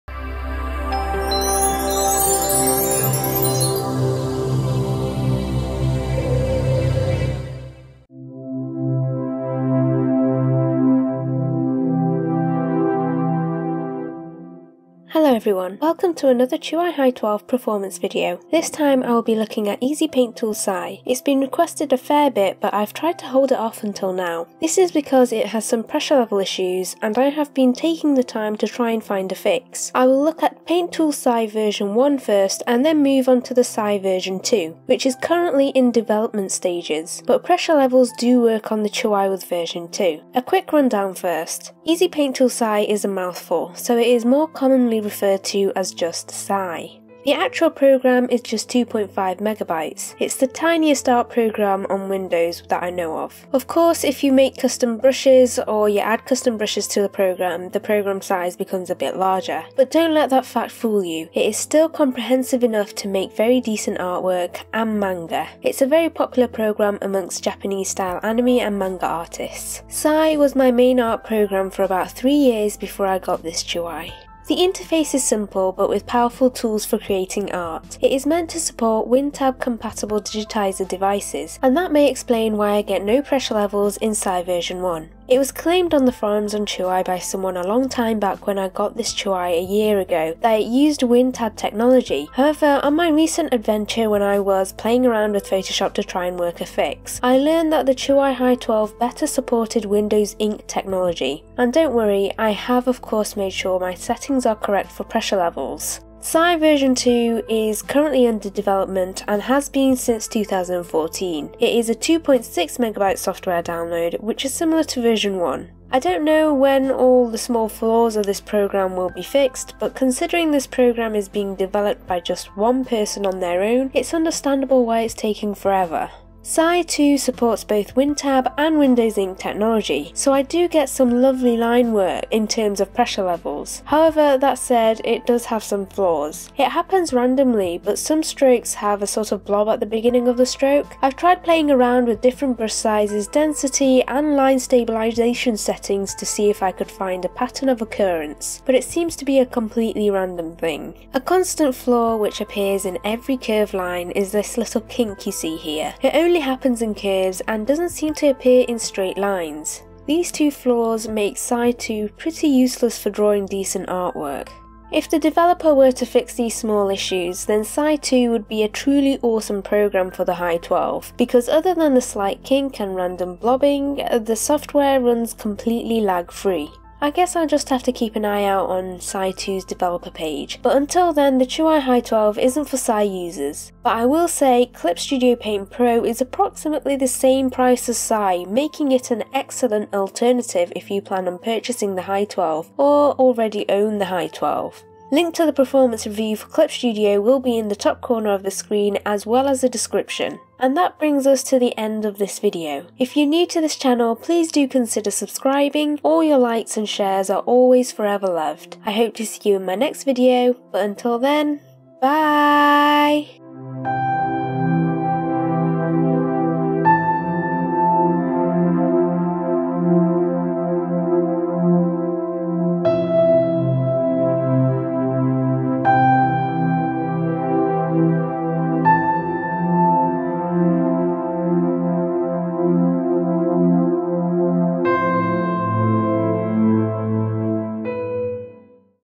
Chuwi Hi12 with Easy Paint Tool Sai
Chuwi Hi12 ★Amazon

A much requested program, Easy Paint Tool Sai. A very small program, yet it has all the tool you need. Very popular amongst anime and manga style artists.